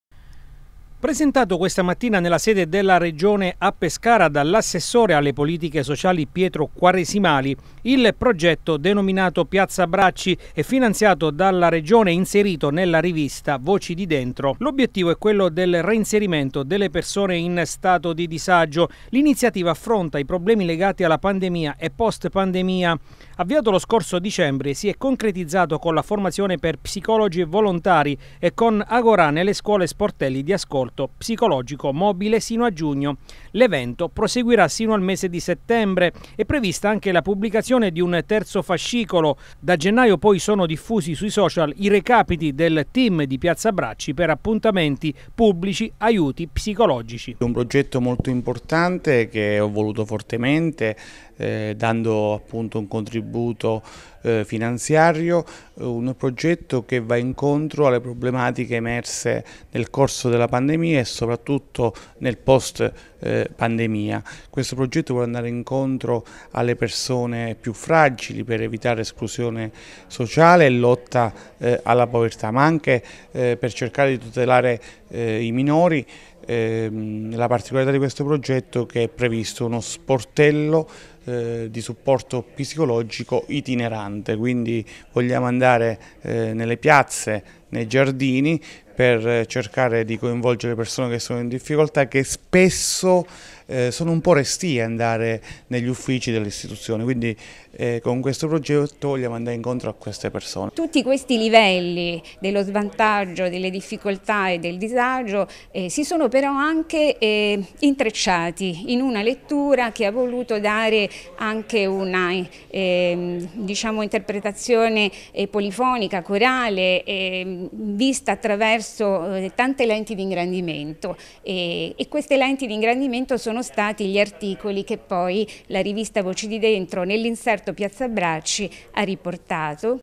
Regione presenta il progetto
L’assessore regionale Pietro Quaresimale ha presentato il secondo numero di piazzabbracci, progetto finanziato dalla Regione, e inserito nella rivista “Voci di dentro” diretta da Francesco Lo Piccolo